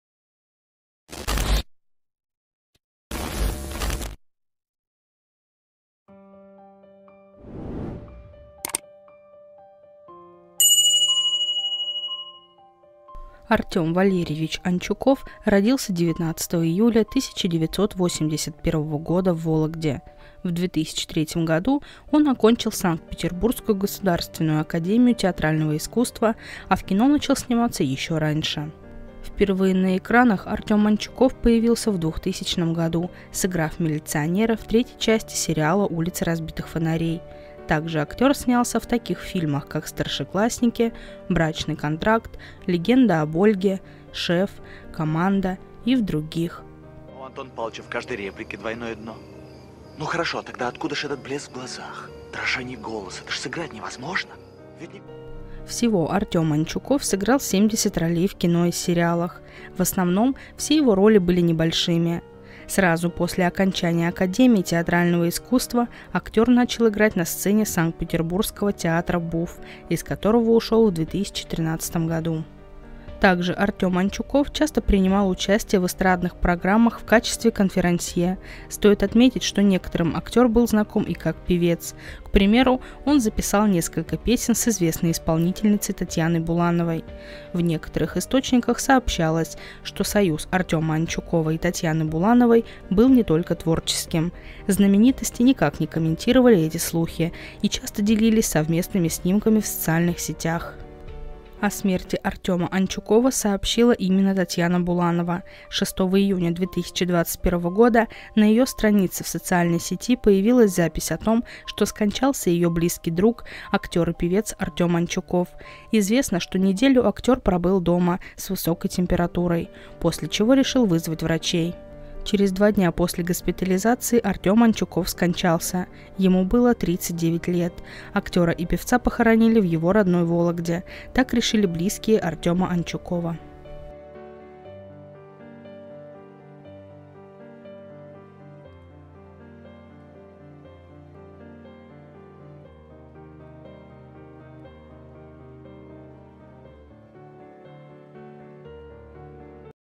УМЕР В 39 ЛЕТ!/ Приписывали РОМАН С БУЛАНОВОЙ// Почему не стало актера и певца Артема Анчукова
( irina_anatolevna_96 )